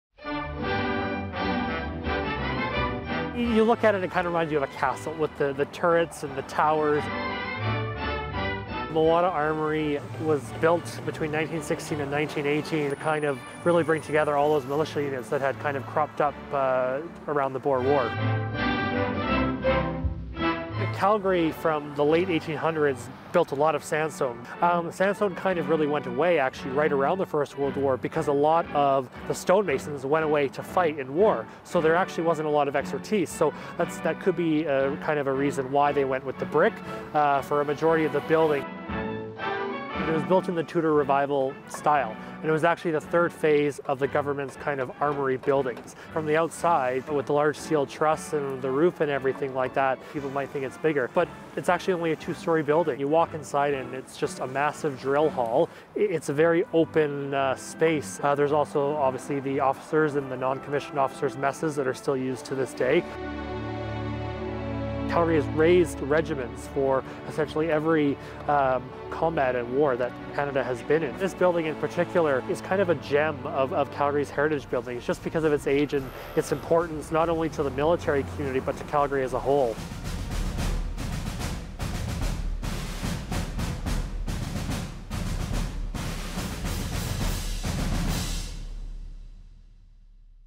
A tour through Calgary's Mewata Armoury
Take a look inside the Mewata Armoury in downtown Calgary and the rich history it holds.